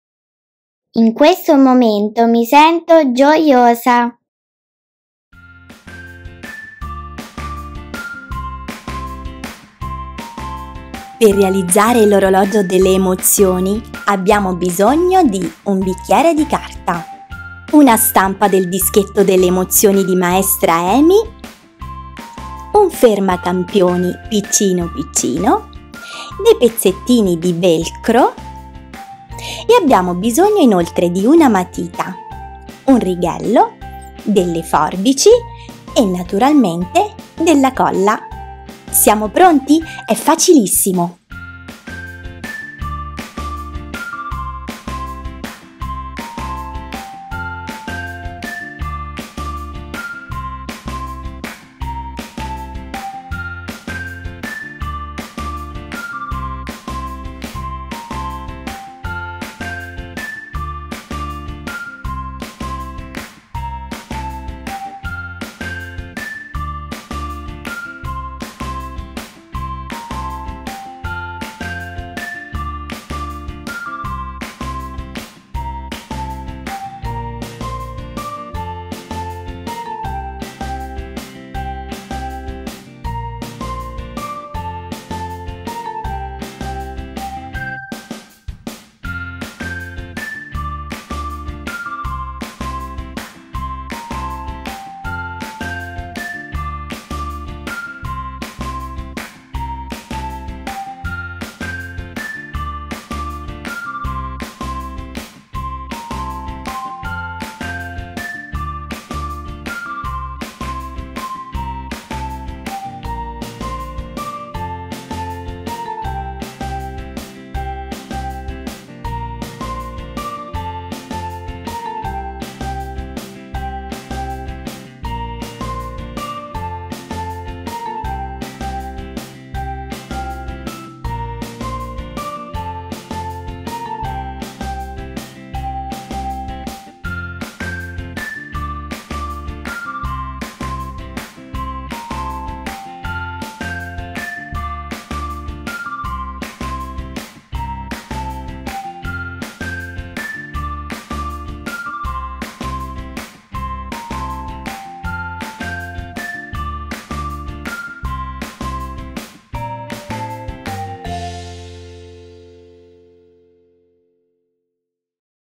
♻️ Laboratorio di riciclo creativo L'OROLOGIO SEGNA-EMOZIONI • PDF in descrizione
In questo momento di che emozione sei?

Dopo un percorso sulle emozioni, in cui i bambini e le bambine imparano a riconoscerle e a nominarle, sarebbe carino realizzare questo orologio.

MATERIALI:
- bicchiere di carta
- fermacampione
- velcro
- dischetto delle emozioni Maestra Emy (qui in basso)

# insideout2